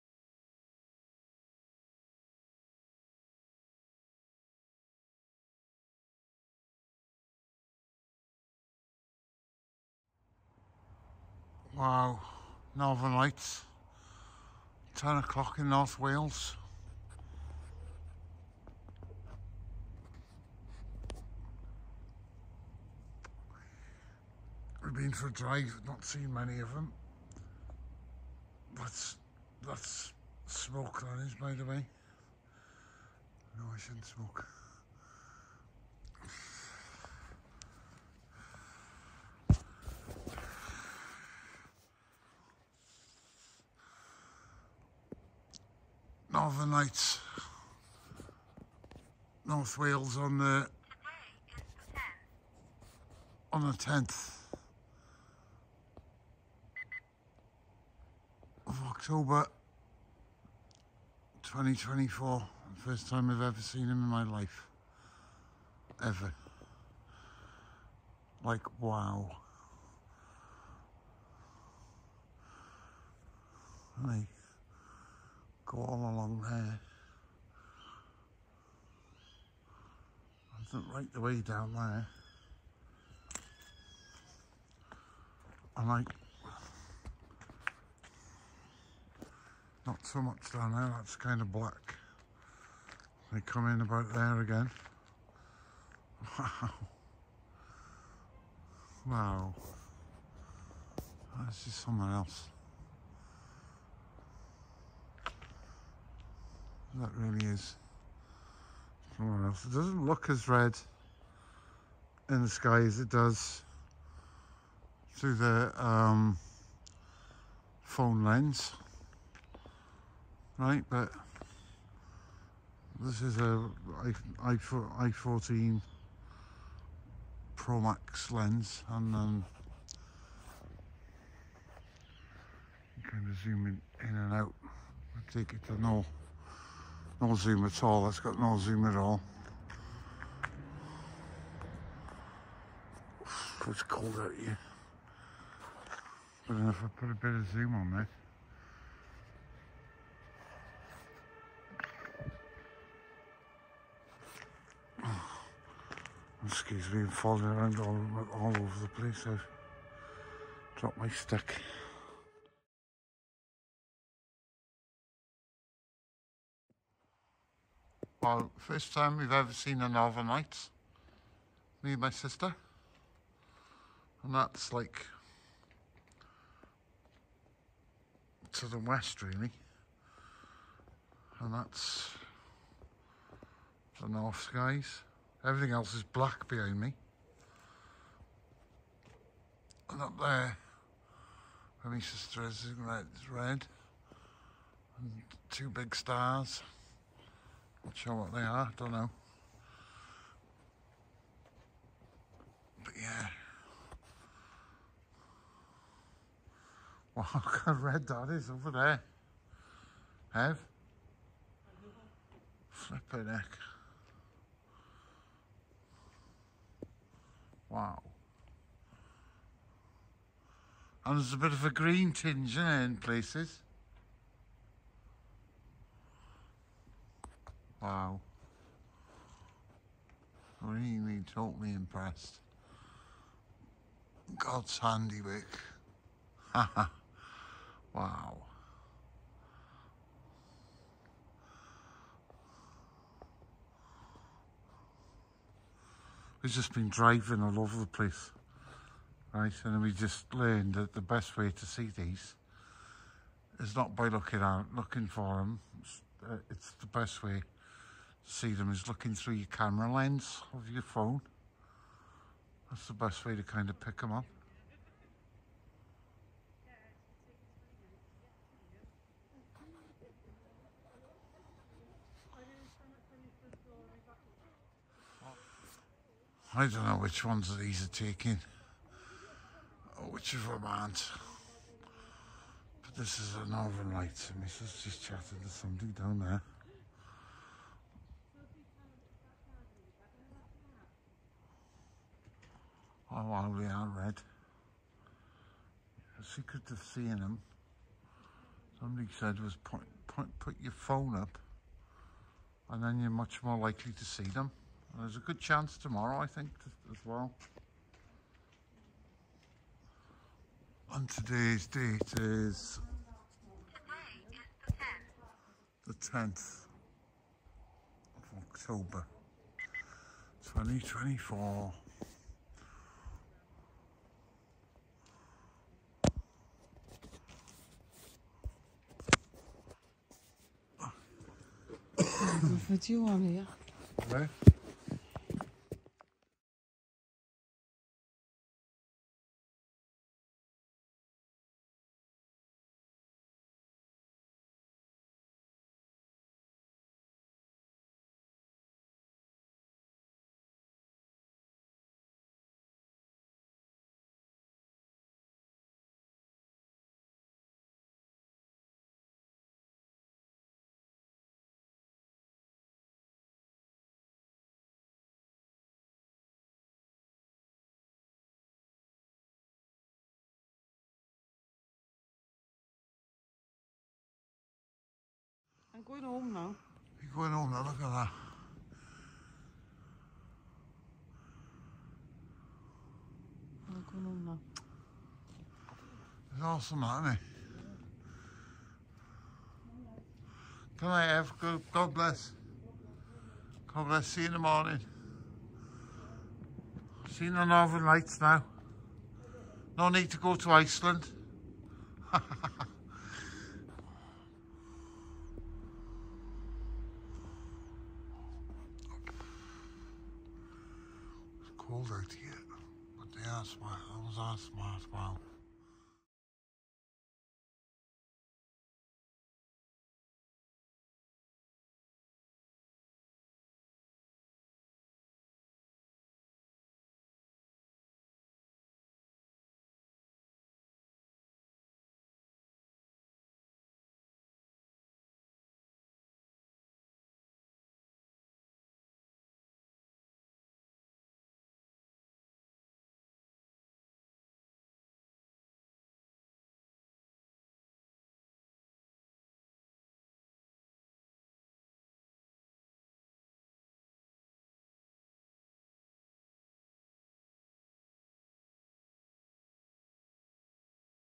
Aurora Borealis/Northern Lights- From The Father of Lights.North Wales 🏴󠁧󠁢󠁷󠁬󠁳󠁿 October 10 -2024🙌 🙌 🙌
Just awesome! We have seen these in all the years that we have been alive, it was a total privilidge to see, and we didnt have to travel to Greenland , or Alaska or up VERY North so see them, they where visible all over the UK.
The KP Index was well above 5 and the solar flare that hit  Earth ,  a cloudless sky too made conditions ideal.
The heavens declare his righteousness, and all the people see his glory.